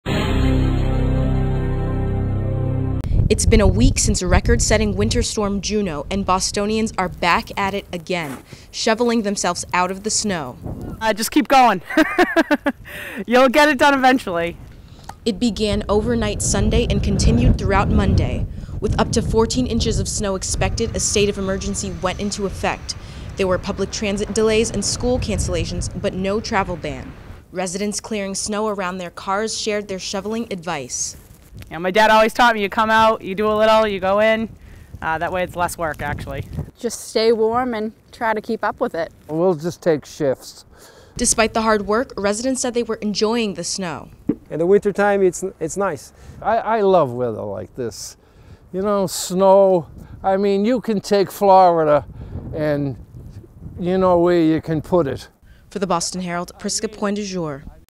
Bostonians give shoveling pointers
A week after a record-setting winter storm, Boston is hit with more snow.